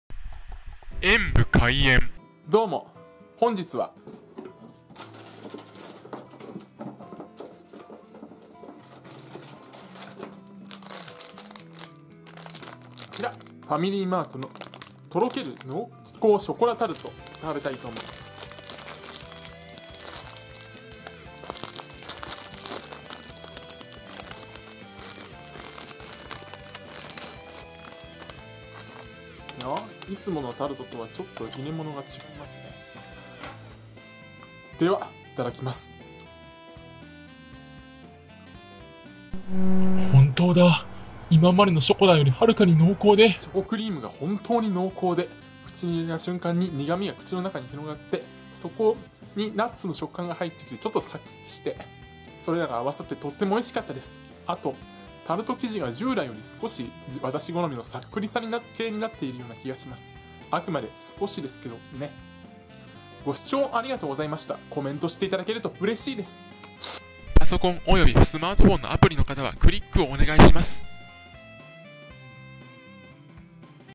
本当に今までとは違います！！ ファミリーマート 濃厚ショコラタルトを食べてみた
ファミリーマート　濃厚ショコラタルトを食べてみました。
ほんつに今までとは濃厚さが違います！！これは……